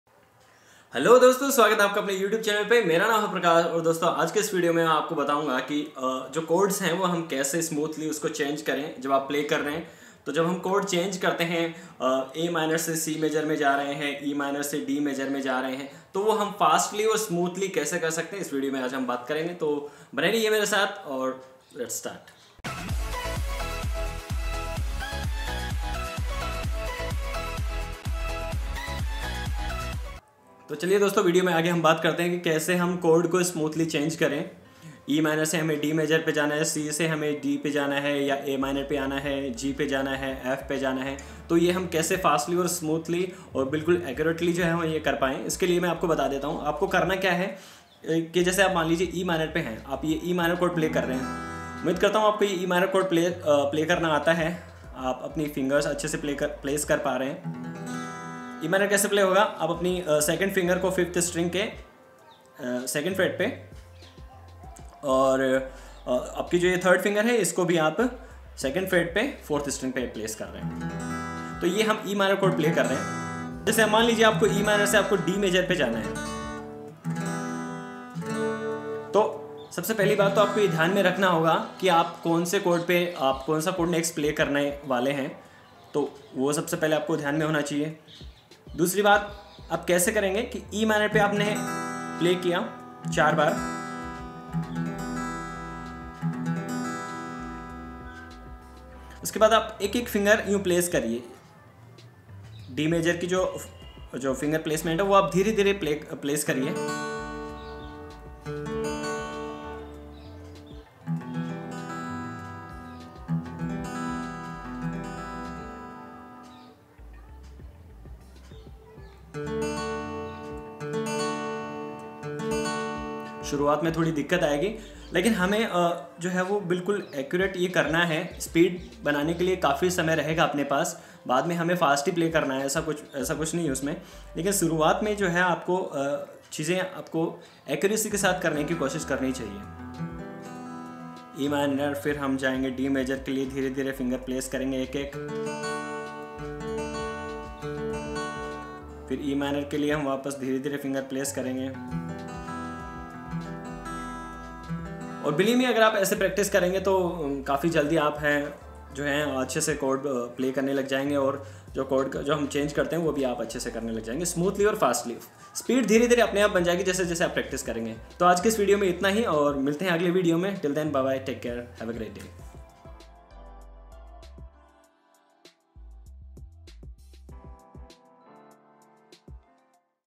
How to change chords on guitar smoothly and quickly while strumming
Hey friends, I hope you all doing a great job learning guitar , practicing daily.
Today in this video I am going to teach you that how we can change chords on guitar during strumming. I will teach you an easy way to improve your chord change speed and accuracy.

Other guitar lessons Videos